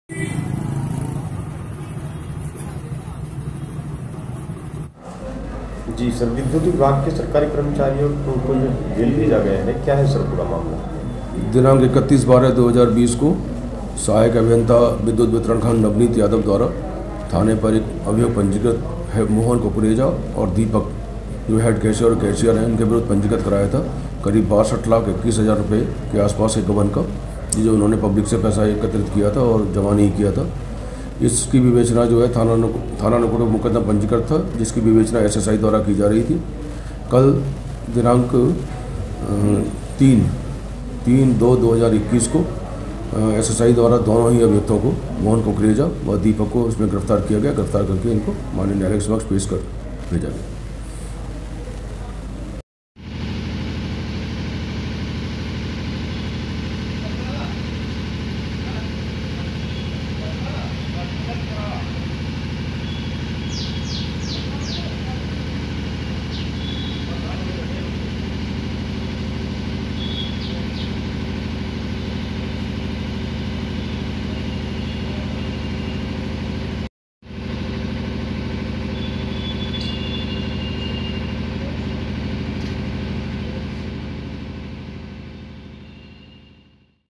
नकुड विद्युत विभाग द्वारा मुख्य के कैसियर व सहायक कैसियर पर 62 लाख से अधिक रुपए के गबन करने का आरोप
ब्रेकिंग सहारनपुर

बिजली घर में लाखों का घोटाला-दो को भेजा जेल।

नगर में तरह-तरह की चर्चाओं का बाजार गर्म

मुख्य कैशियर व सहायक कैशियर पर 62 लाख से अधिक रुपए के गबन का आरोप

एसडीओ गंगोह द्वारा दी गई थी मुख्य कैशियर व सहायक कैशियर के खिलाफ तहरीर

पुलिस ने जांच कर दोनों आरोपियों को दबोचा
62 लाख 27 हजार 263 रुपये गबन करने का आरोप
पुलिस ने गिरफ्तार कर दोनों को भेजा जेल

पुलिस दोनों की कुंडली रही खंगाल

नकुड थाना क्षेत्र के नकुड बिजली घर का पूरा मामला